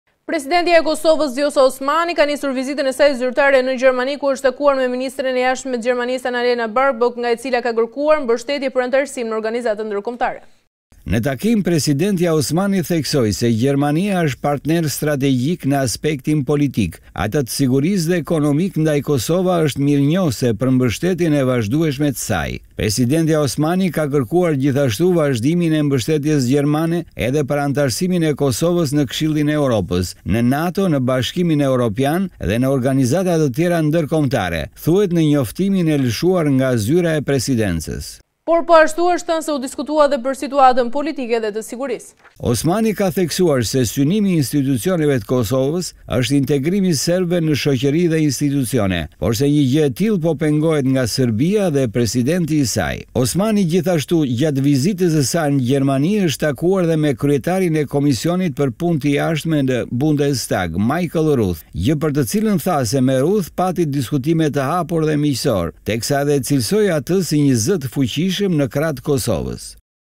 Osmani kërkon mbështetje nga Gjermania/ Ja cfare po ndodh | Aktualitet
AKUZA TE RENDA NDAJ VUCIC
GODITET DYSHEN VUCIC DHE DODIK
LAJCAK VE NE ZBATIM PLANIN B
OSMANI KERKON NDIHME NGA GJERMANIA
RRITEN TENSIONET NE KOSOVE?

𝐀𝐮𝐭𝐨𝐫 : NTV